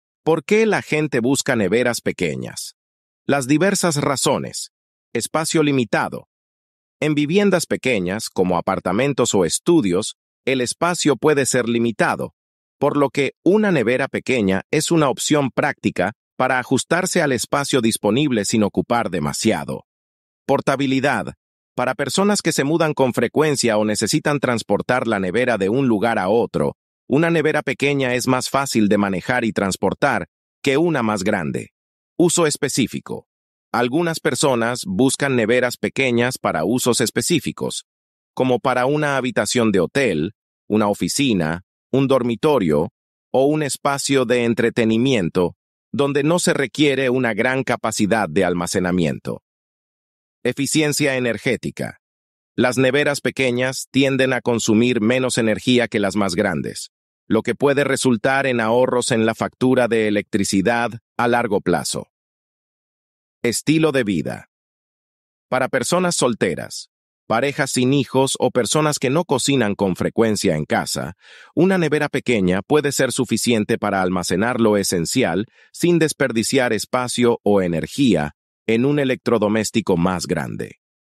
La gente busca neveras pequeñas por diversas razones:
Espacio limitado: En viviendas pequeñas, como apartamentos o estudios, el espacio puede ser limitado, por lo que una nevera pequeña es una opción práctica para ajustarse al espacio disponible sin ocupar demasiado.
Portabilidad: Para personas que se mudan con frecuencia o necesitan transportar la nevera de un lugar a otro, una nevera pequeña es más fácil de manejar y transportar que una más grande.
Uso específico: Algunas personas buscan neveras pequeñas para usos específicos, como para una habitación de hotel, una oficina, un dormitorio o un espacio de entretenimiento, donde no se requiere una gran capacidad de almacenamiento.
Eficiencia energética: Las neveras pequeñas tienden a consumir menos energía que las más grandes, lo que puede resultar en ahorros en la factura de electricidad a largo plazo.
Estilo de vida: Para personas solteras, parejas sin hijos o personas que no cocinan con frecuencia en casa, una nevera pequeña puede ser suficiente para almacenar lo esencial sin desperdiciar espacio o energía en un electrodoméstico más grande.
En resumen, las neveras pequeñas son populares debido a su practicidad, versatilidad y eficiencia en espacios y situaciones específicas.